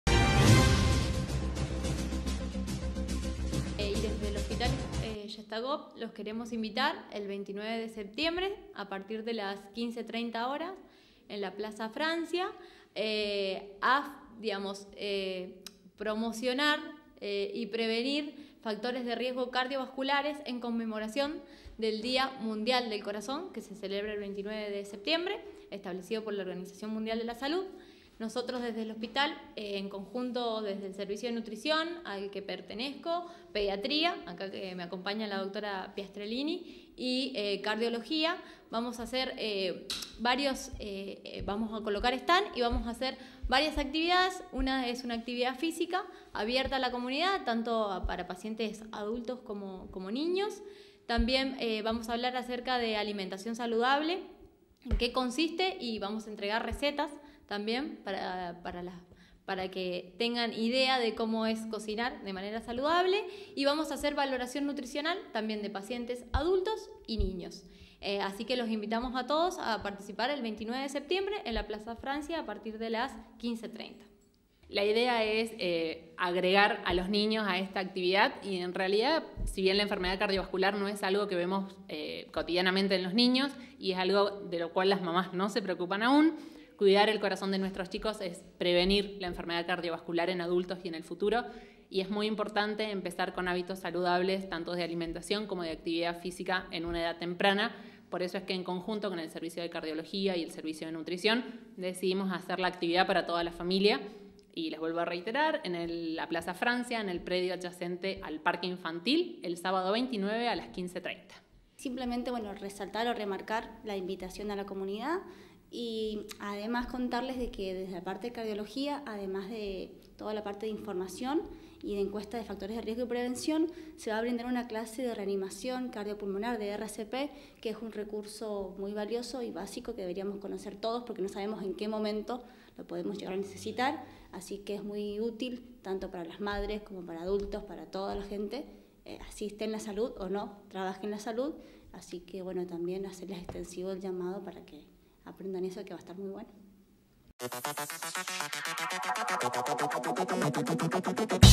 MI CORAZÓN, TU CORAZÓN
Este fin de semana se desarrollará una actividad en la Plaza Francia para promocionar acciones saludables en relación al corazón.